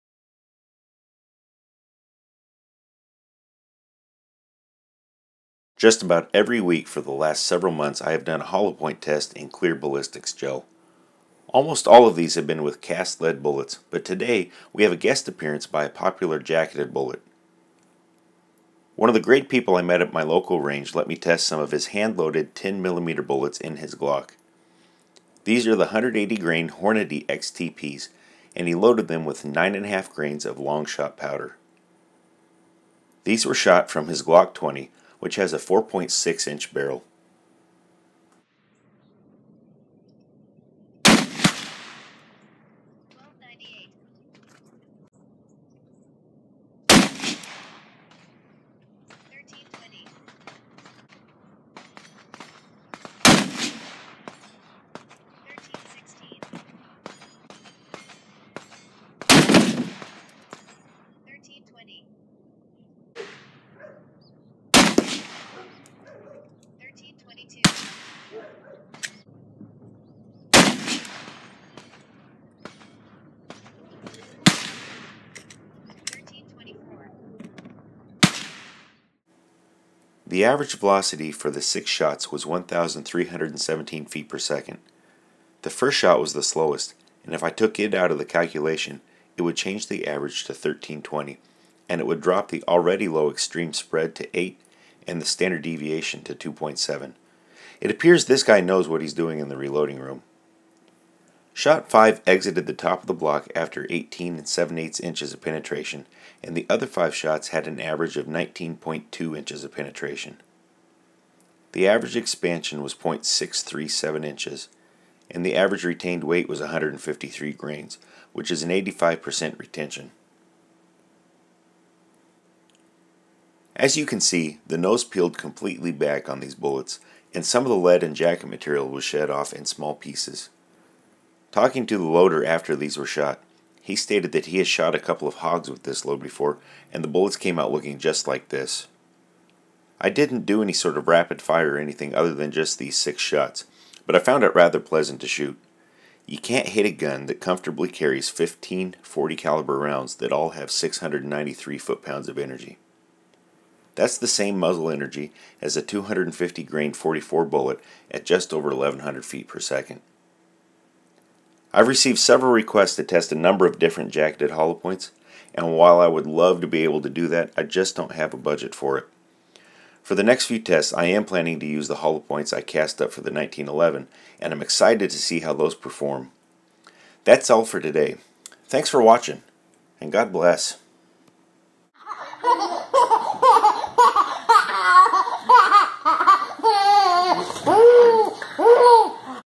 Hornady 10mm 180 Grain XTP Ballistics Gel Test
I tested the 10mm 180 grain Hornady XTP shot from a Glock 20 into ballistics gel.  These were loaded with 9.5 grains of Hodgdon Longshot powder.